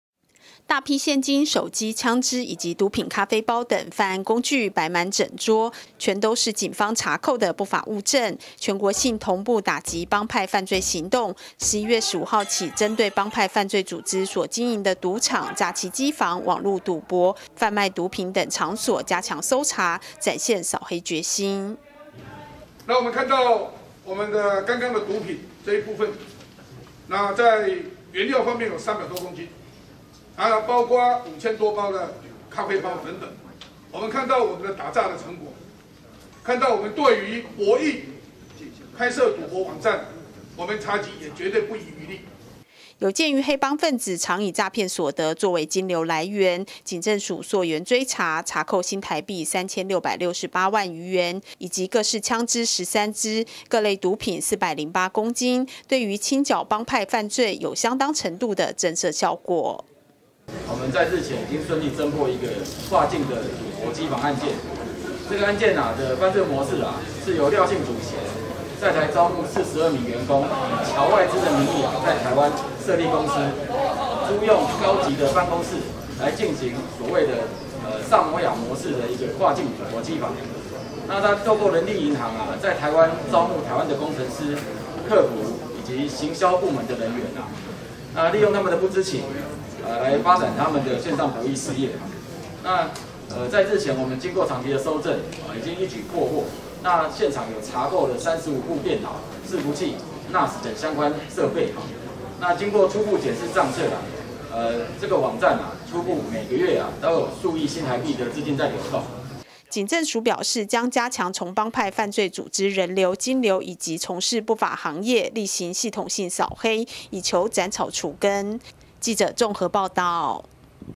全國 掃黑|全國掃黑行動 逮182人 斷黑幫金流
近來發生多起街頭聚眾鬥毆案件，造成治安疑慮，警政署加強全國性同步打擊幫派犯罪行動，出動警力逾3,000人次，查獲25個幫派犯罪組織，幫派成員182人，並加強搜查幫派組織經營的不法場所，展現掃黑決心。